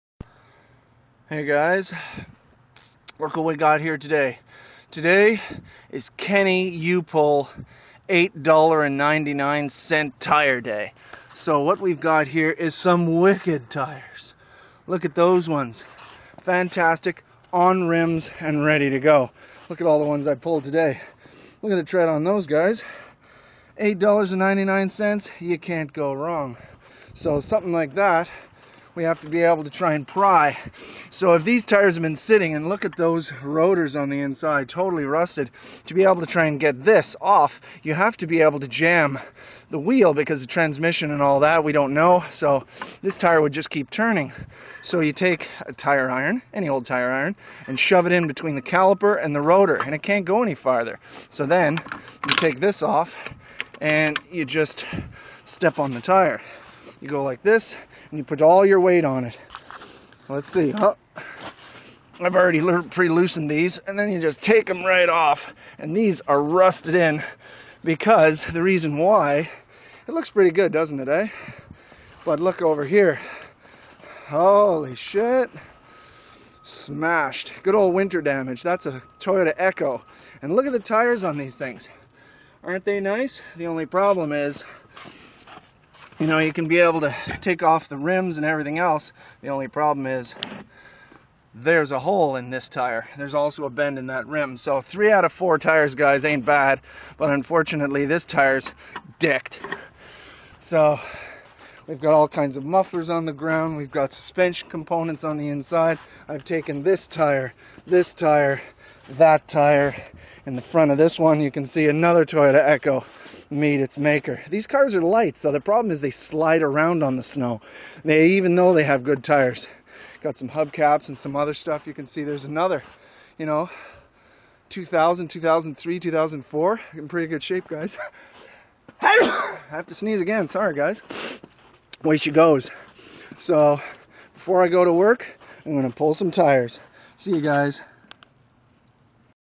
kenny u pull $8.99 tire day
used tires  at half price  ...steel rims 4.99$ and tires 8.99..i picked up 2 for my ram and 5  for freinds insted of 6.. 1 tire was flat